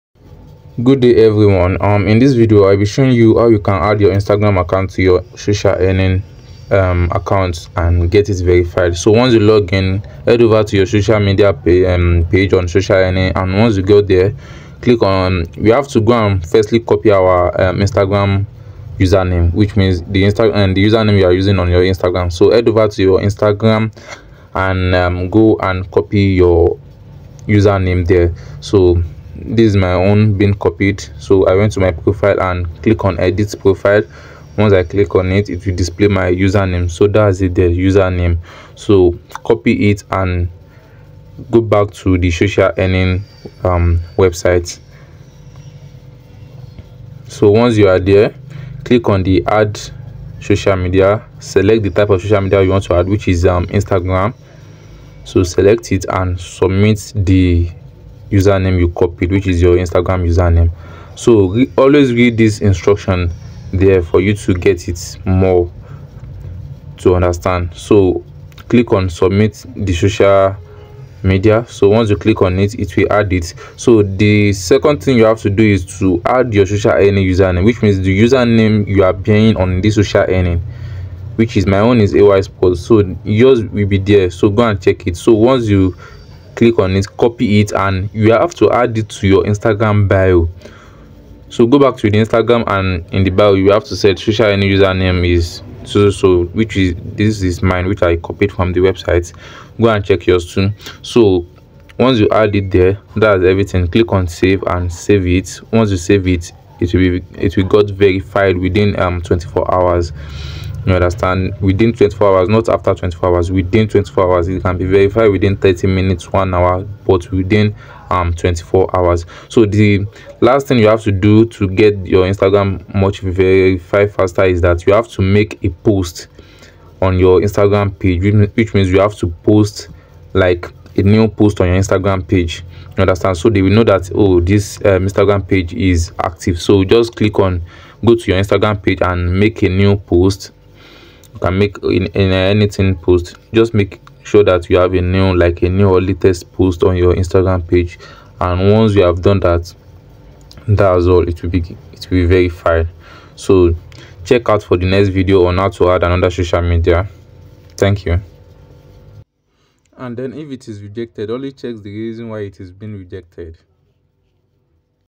How to add your Instagram account on SocialEarning
In this video i will be showing you how to add your Instagram accounts to socialearning.org. so that you can perform Instagram tasks.